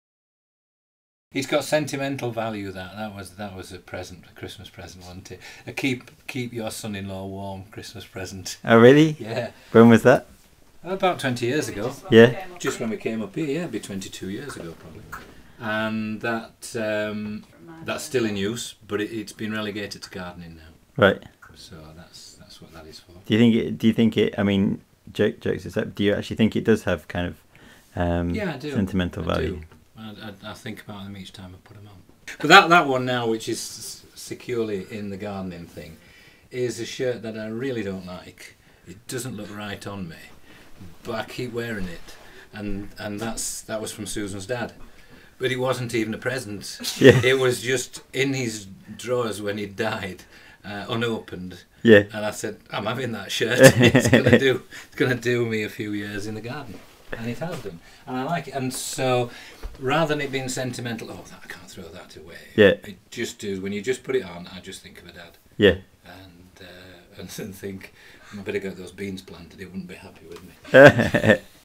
Interview with Lucy Martin.mov
'Flight-drawing interpretations' A project with London College of Fashion, UAL and The National Gallery, London.